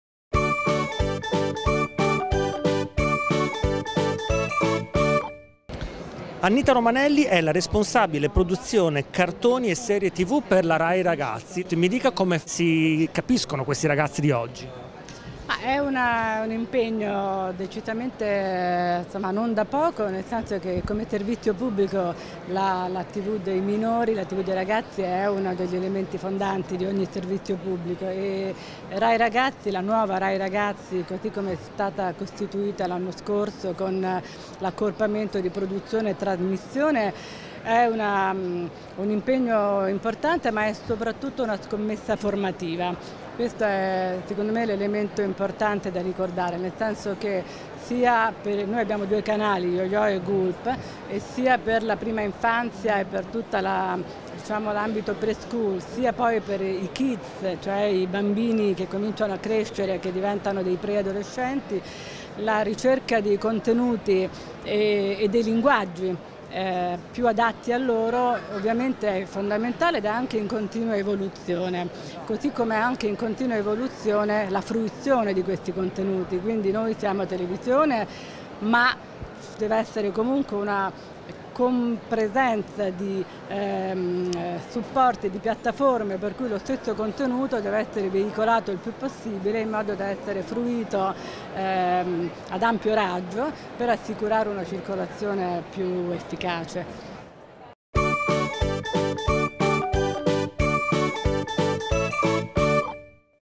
BLTF18 | Rai Ragazzi scommette su contenuti di qualità per bambini e ragazzi
I programmi tv di qualità per bambini e ragazzi è stato il tema del convegno che si è tenuto al  Bologna Licensing Trade Fair, edizione 2018. Da una parte i broadcaster tematici che propongono palinsesti di appeal e dall'altra licensor che creano programmi per tenere alta l'attenzione dei giovanissimi sulle loro properties. Tra i relatori, Annita Romanelli, Responsabile Produzione Cartoni e Serie TV di Rai Ragazzi.  - video by Publivideo2